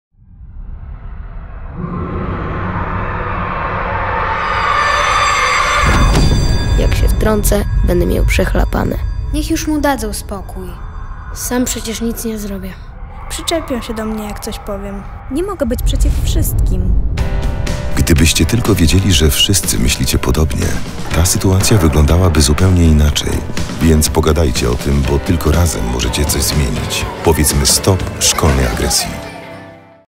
Kampania społeczna "Powiedzmy STOP szkolnej agresji" (2018)
Spot kampanii społecznej Rzecznika Praw Dziecka Marka Michalaka "Powiedzmy STOP szkolnej agresji".
„Powiedzmy STOP szkolnej agresji”  ma na celu zmianę postaw młodzieży wobec przemocy oraz zmniejszenie zachowań agresywnych. Za strategię oraz kreację kampanii odpowiada agencja Telescope. W ramach akcji „Powiedzmy STOP szkolnej agresji” dom produkcyjny Papaya Films przgotował spot telewizyjny.

–  Przemoc rówieśnicza dotyczy wszystkich. Praprzyczyną przemocy rówieśniczej jest przemoc, jakiej dziecko doświadczyło wcześniej. Dzieci uczą się obserwując nas, dorosłych. Zachowania agresywne w szkole względem rówieśników mogą być odzwierciedleniem sytuacji, z jakimi dziecko styka się poza szkołą. Czasem może to być próba odreagowania trudnej sytuacji domowej. Dlatego kluczowym jest obserwacja, rozmowa, empatia, gotowość do działania, pomocy. – mówi Rzecznik Praw Dziecka Marek Michalak.